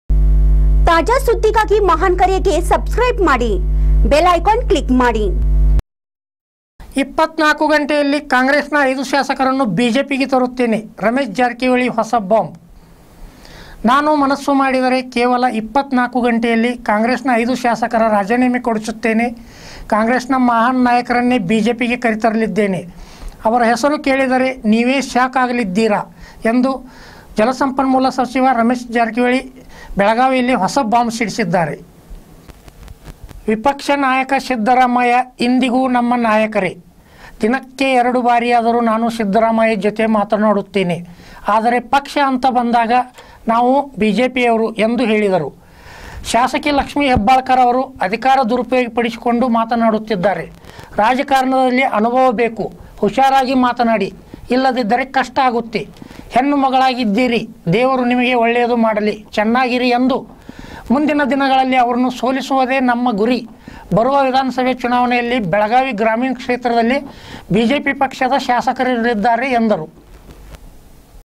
24 ಗಂಟೆಯಲ್ಲಿ ಕಾಂಗ್ರೆಸ್ ನ 5 ಶಾಸಕರನ್ನು ಬಿಜೆಪಿಗೆ ತರುತ್ತೇನೆ: ರಮೇಶ ಜಾರಕಿಹೊಳಿ ಹೊಸ ಬಾಂಬ್..!!!
24 ಗಂಟೆಯಲ್ಲಿ ಕಾಂಗ್ರೆಸ್ ನ 5 ಶಾಸಕರನ್ನು ಬಿಜೆಪಿಗೆ ತರುತ್ತೇನೆ: ರಮೇಶ ಜಾರಕಿಹೊಳಿ ಹೊಸ ಬಾಂಬ್..!!!